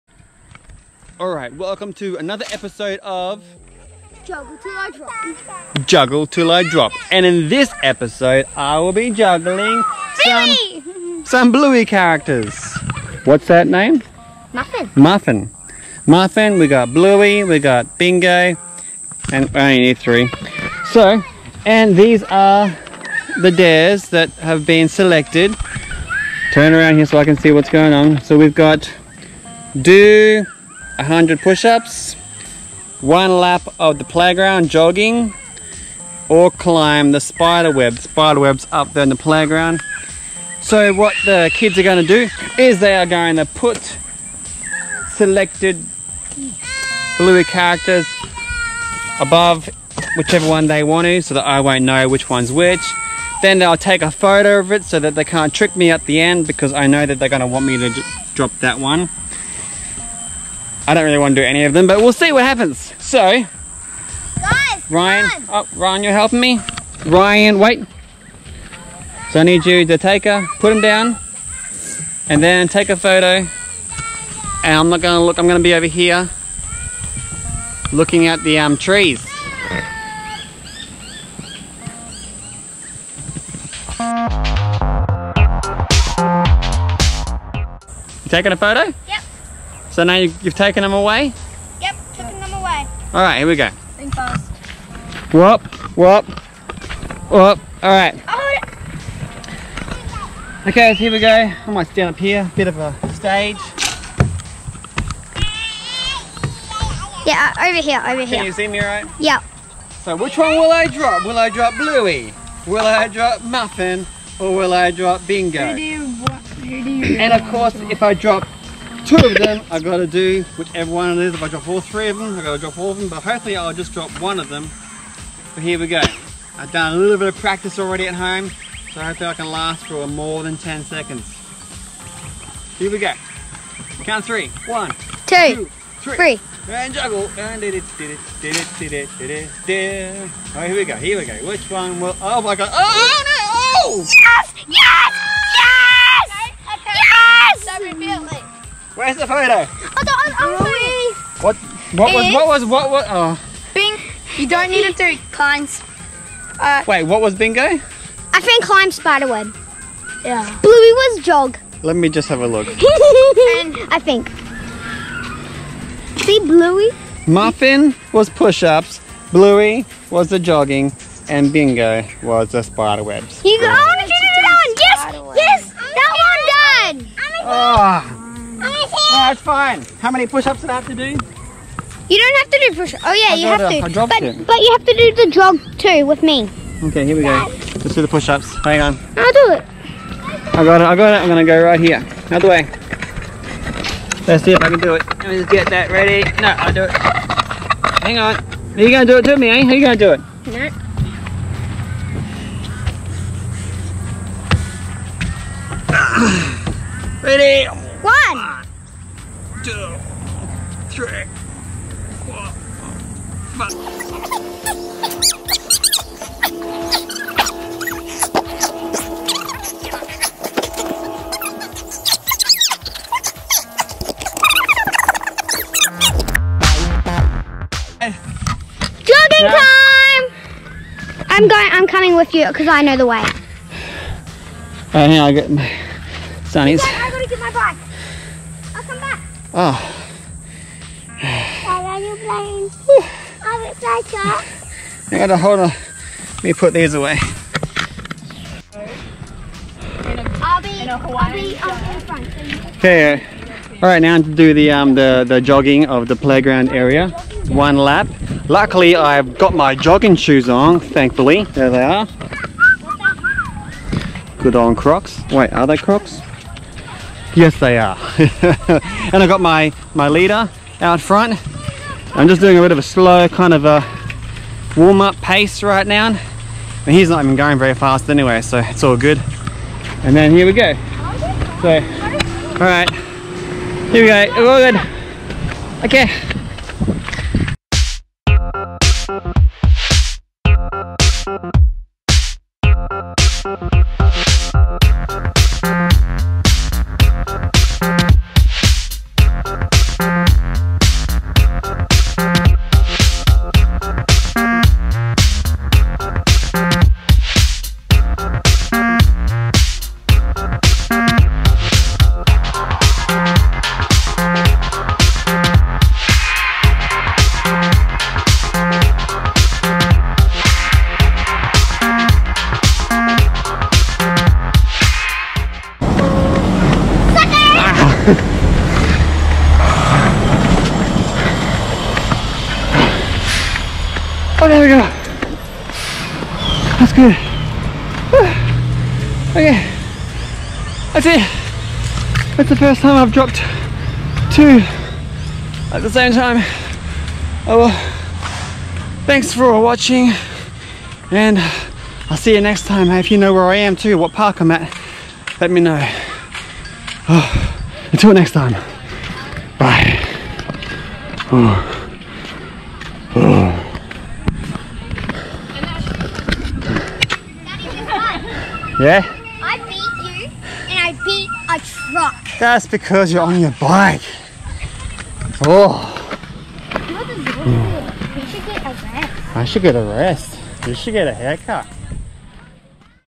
Juggle til I Drop - the Bluey edition
Welcome to another episode of "Juggle til I Drop". In this episode I juggle characters from the kids show BLUEY. Each character is connected to a "dare" that I have to do if I drop that character. 1) Do 100 pushups 2) Jog a lap of the playground 3) Climb the Spider-web play equipment.